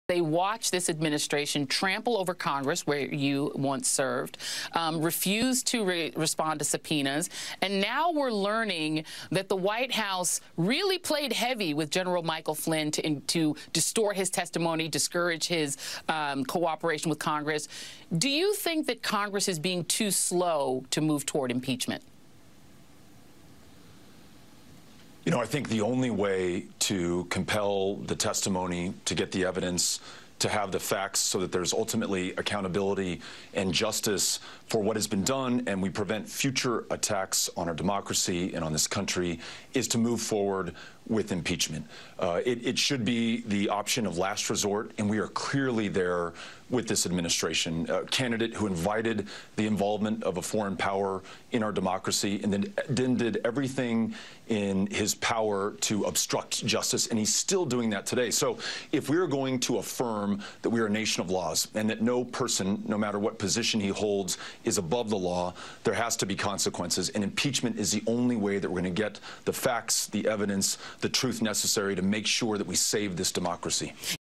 Beto O'Rourke: Trump Is Still Obstructing Justice And Must Be Impeached
Democratic presidential candidate Beto O'Rourke said that Trump is still obstructing justice and the only way to get the evidence is to impeach him.